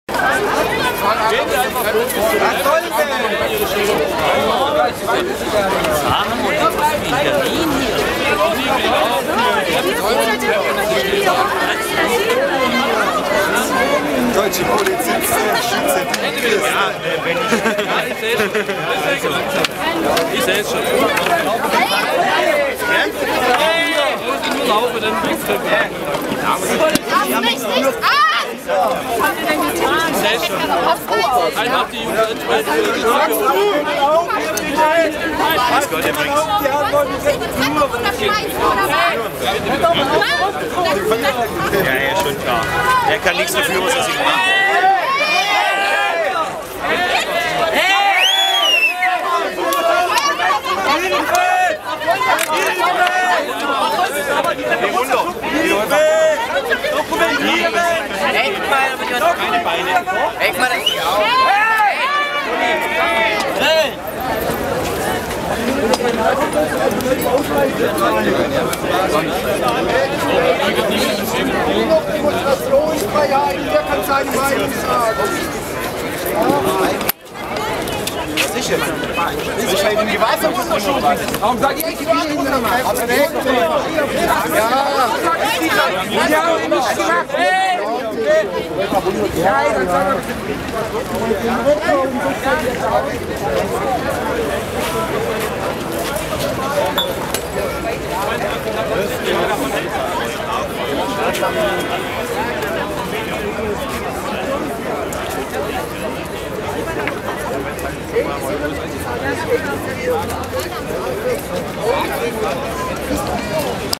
CSD 2012: Polizei schubst Pius-Brüder-Gegner
Fünf religiöse Fanatiker der Piusbrüder (am Ende kurz zu sehen) demonstrieren gegen den Christopher Street Day (CSD) 2012 in Stuttgart, ein Haufen Gegen-Gegendemonstranten protestieren. Gerangel und Geschubse mit der Polizei.
Was für ein Aufstand, wegen ein paar Demonstranten. Lasst doch beide Seiten demonstrieren, Polizeikette dazwischen, fertig. Das Geschubse erscheint mir eher unnötig.